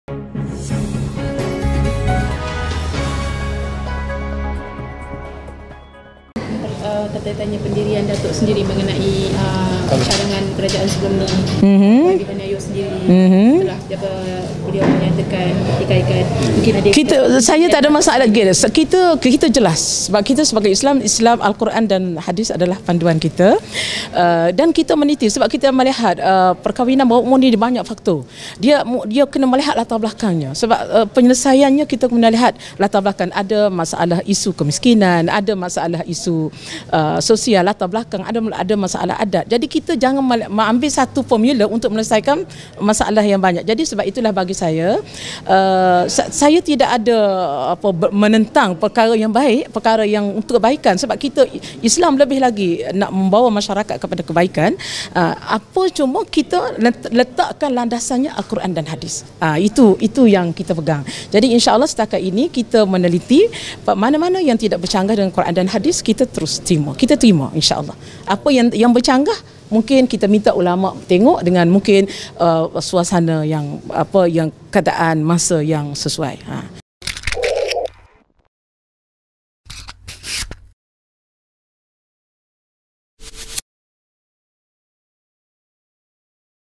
TERKINI : Perkahwinan Bawah Umur Perlu Diteliti - Zailah Yusoff
Timbalan Menteri Wanita dan Keluarga Datuk Siti Zailah Mohd Yusoff terbuka dengan dasar sedia termasuklah usaha kerajaan terdahulu untuk melaksanakan Pelan Strategi Kebangsaan Bagi Menangani Punca Perkahwinan Bawah Umur.

Walaubagaimanapun beliau berkata apa juga dasar haruslah tidak bercanggah dengan landasan Islam.

Justeru itu ujarnya perkara tersebut memerlukan penelitian dan kepakaran khsusnya melihat ruang untuk proses penambahbaikan dilakukan.

Jelasnya, isu had umur perkahwinan melibatkan pelbagai perkara termasuklah latar belakang, adat, dan taraf hidup.